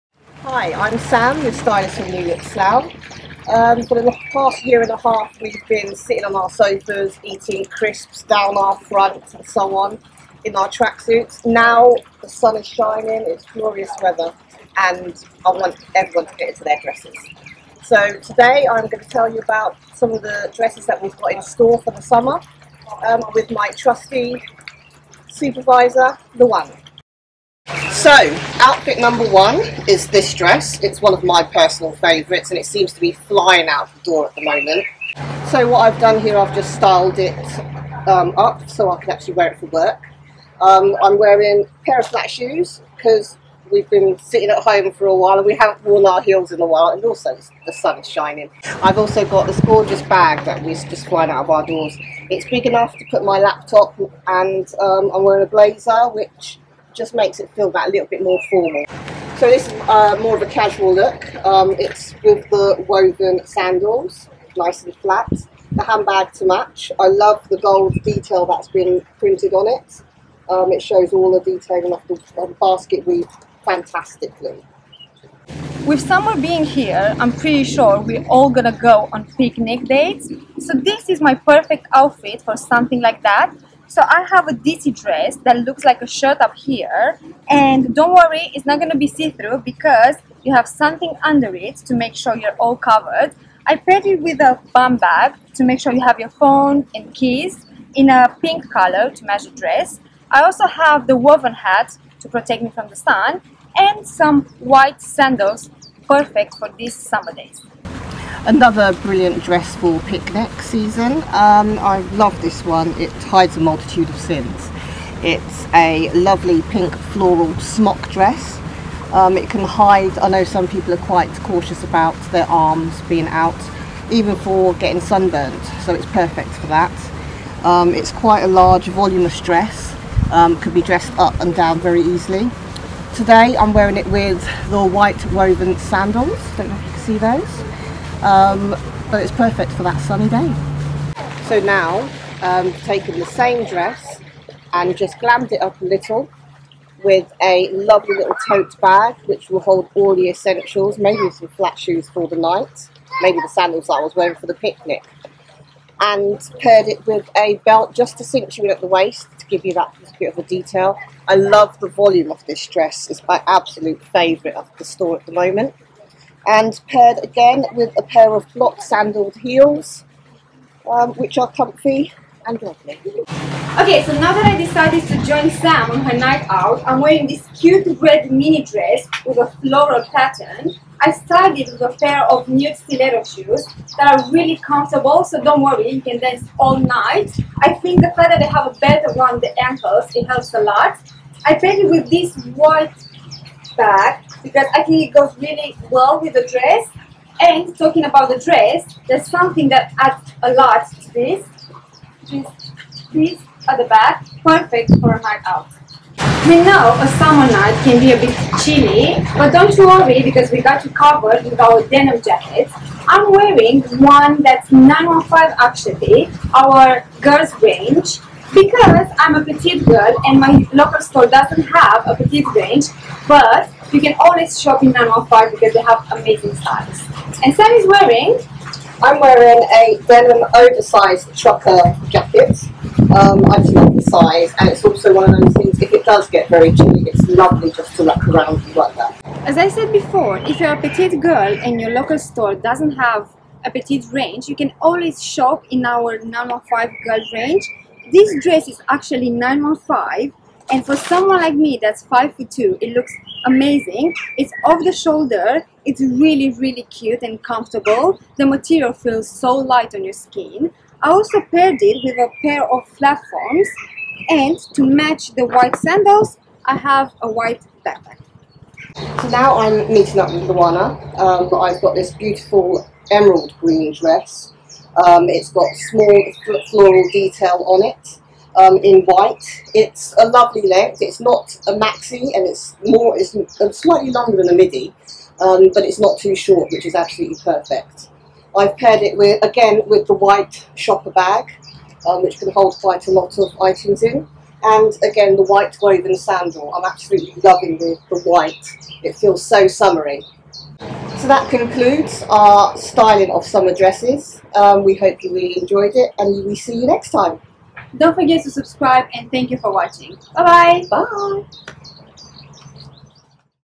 Our dresses range styled by Sam and Luana
Stylist Sam and supervisor Luana from Slough store showing you ways to wear our beautiful dresses. Available online and in store.

For styling advices from Luana and Sam visit New Look Slough.

Dresses

• Red Floral Puff Sleeve Open Back Mini Dress (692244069)

• Girls Light Green Floral Ruffle Shirred Midi Dress (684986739)

• Green Ditsy Floral Tie Front Midi Slip Dress (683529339)

• Freya Pink Long Sleeve Dress (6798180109)

Shoes

• Ria Nude Stiletto Heels (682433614)

Bags

Jackets

Accessories